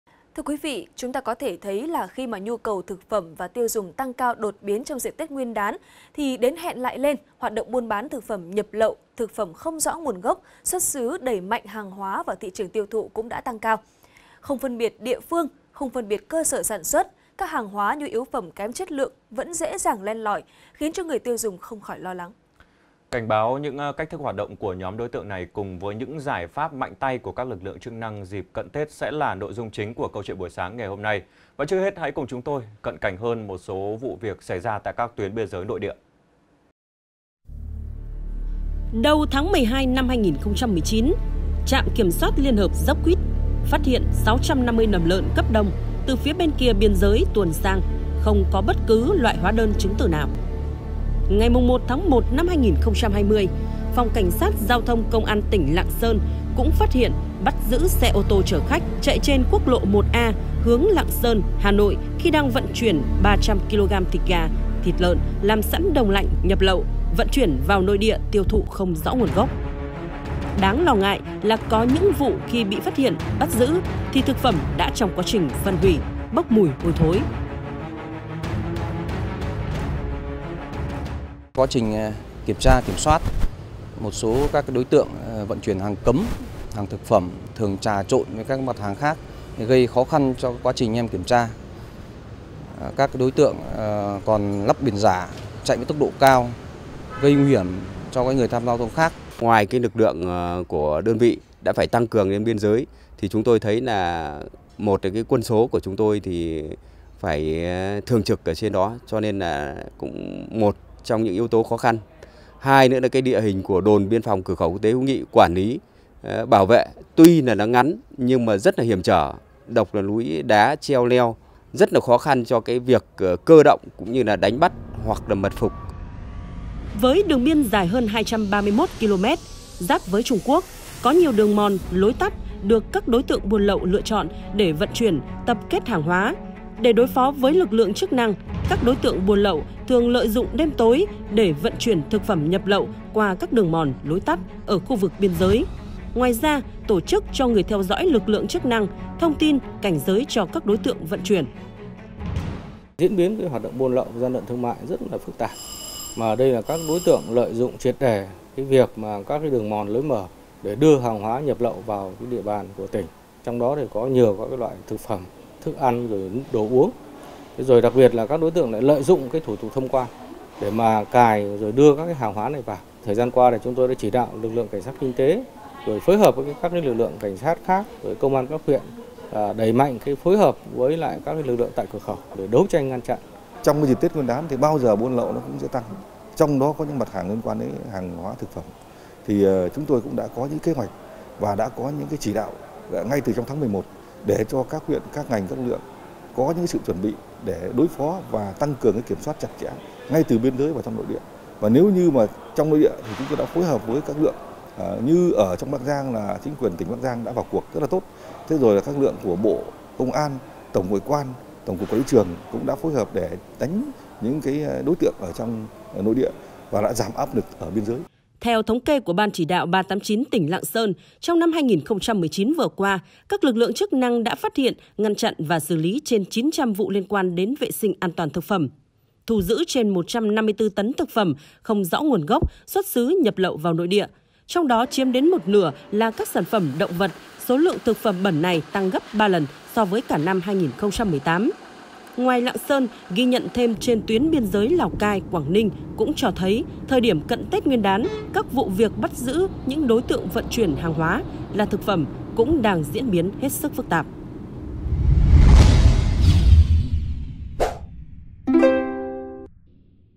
An toàn thực phẩm ngày Tết - Nỗi lo từ biên giới| VTC14
VTC14 |Đến hẹn lại lên  hoạt động buôn bán thực phẩm nhập lậu, thực phẩm không rõ nguồn gốc, xuất xứ đẩy mạnh hàng hóa vào thị trường tiêu thụ cũng tăng cao. Không phân biệt địa phương, không phân biệt cơ sở sản xuất, các hàng hóa nhu yếu phẩm kém chất lượng vẫn dễ dàng len lỏi, khiến người tiêu dùng không khỏi lo lắng